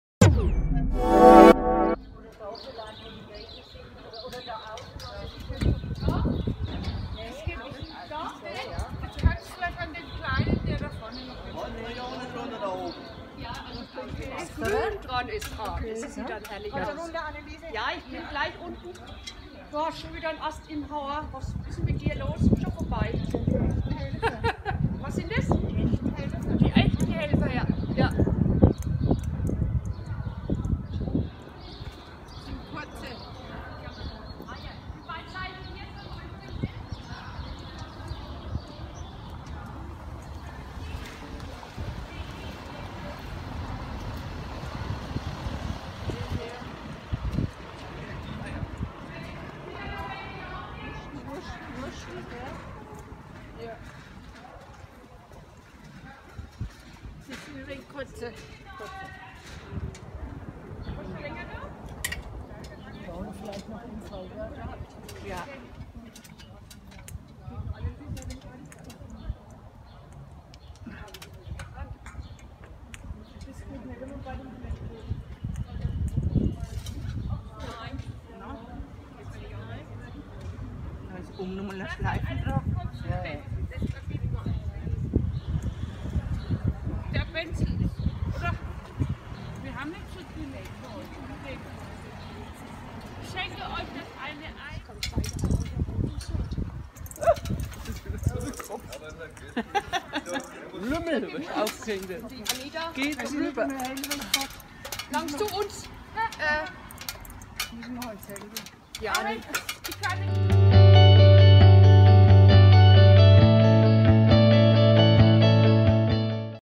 Schmücken des Osterbrunnens
Zehn Frauen schmücken den Marktplatzbrunnen in Gerolzhofen mit 1000 bunten Eiern.